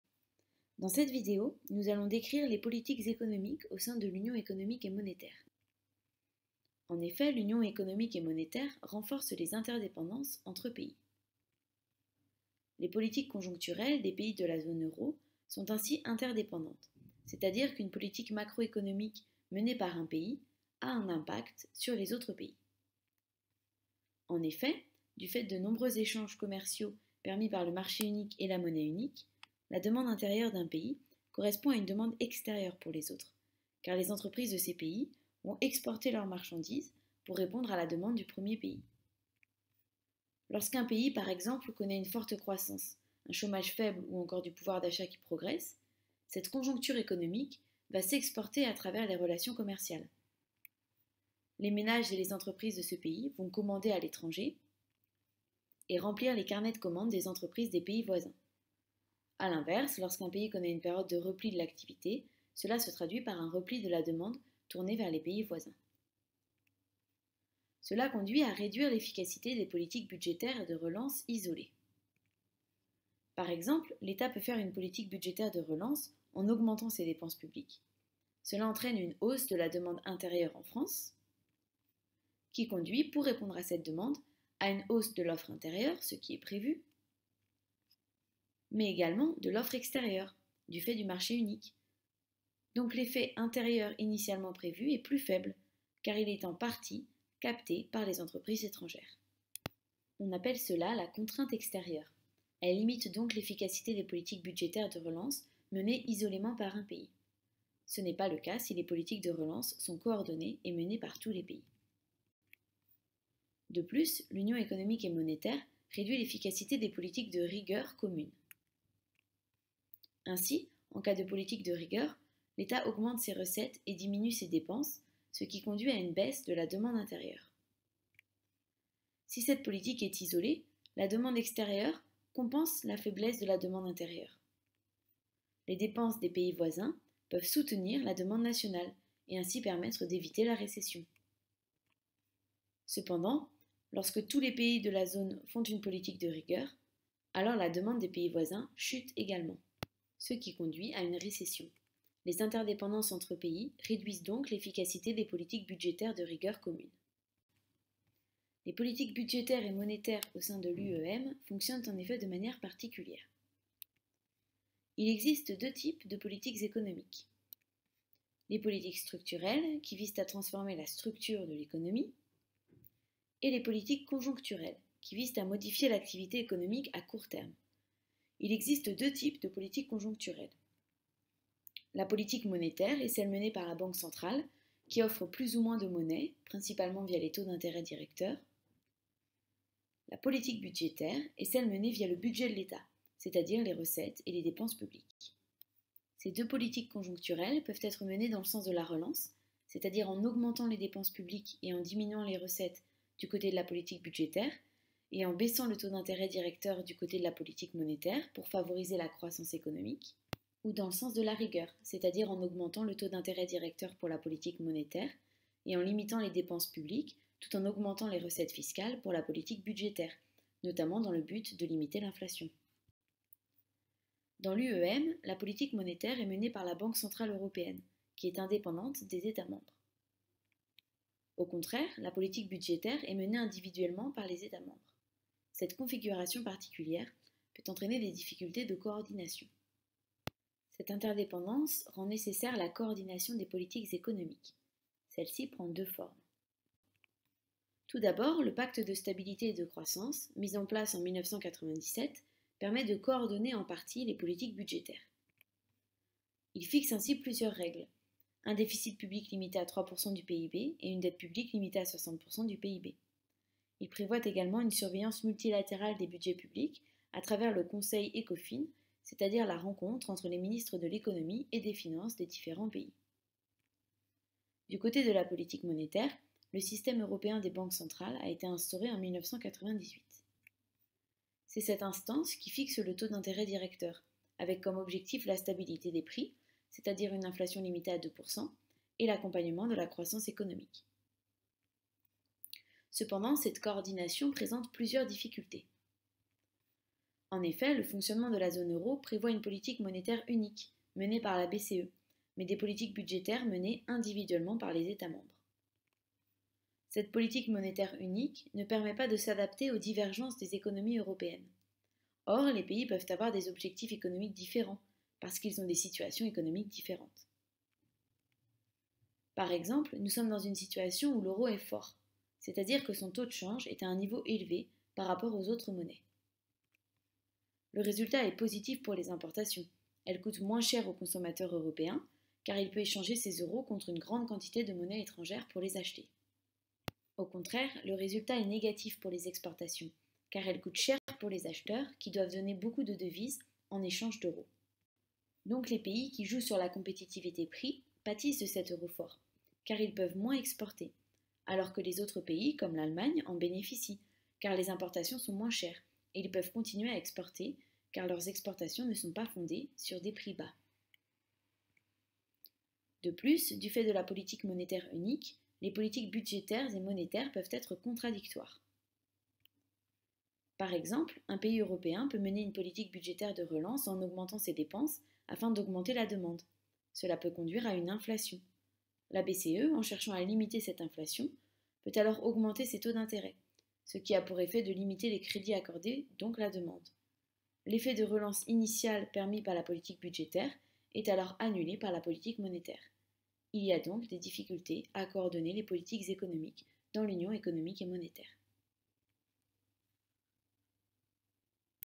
Les politiques économiques au sein de l'Union économique et monétaire
Chapitre : Quelle est la place de l'Union européenne dans l'économie globale ?

Partie 2 : Les politique économiques au sein de l'Union économique et monétaire